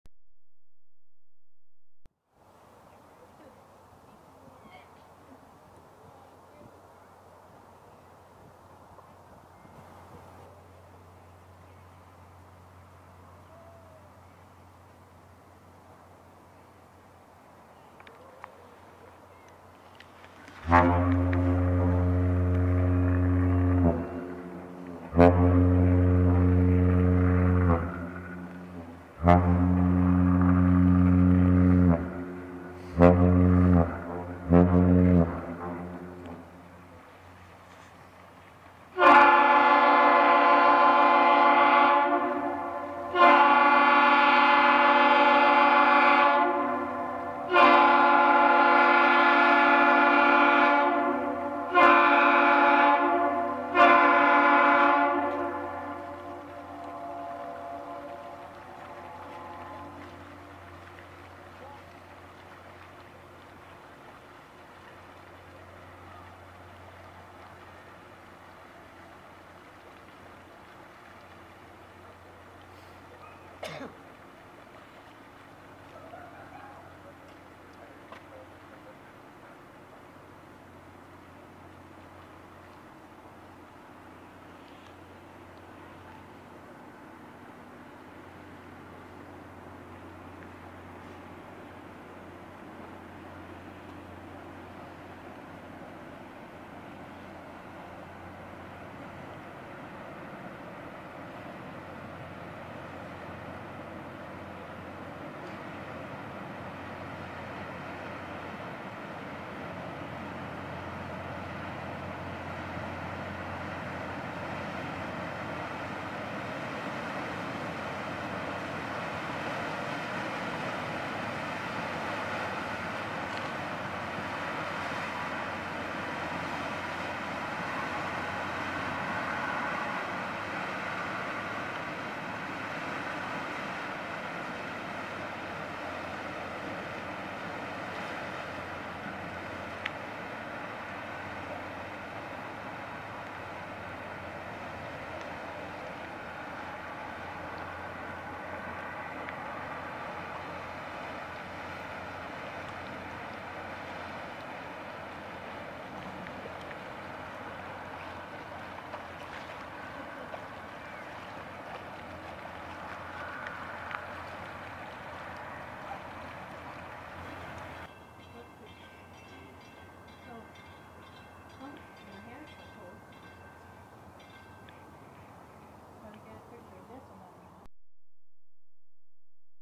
JOHN G MUNSON and the Mellow Master
JOHN G MUNSON arrives in Duluth with limestone.  They will eventually load iron ore pellets in Two Harbors Minnesota.

Name of ship  JOHN G.MUNSON
Former names  JOHN G MUNQ N H (2018, United States (USA))
Operating status  Active
Flag  United States (USA)
Length  234 m  or 768 feet
Breadth  21 m  or 72 feet
Year of build  1952
Builder  MANITOWOC SHIPBUILDING - MANITOWOC WI, U.S.A.
Classification society  AMERICAN BUREAU OF SHIPPING
Home port  DULUTH MN
Owner  GREAT LAKES FLEET - DULUTH MN, United States (USA)
Manager  GREAT LAKES FLEET - DULUTH MN, United States (USA)
Description  JOHN G.MUNSON is a Bulk carrier built in 1952 by MANITOWOC SHIPBUILDING - MANITOWOC WI, U.S.A.. Currently sailing under the flag of United States (USA). Formerly known as JOHN G MUNQ N H. Gross tonnage is 15179 tons.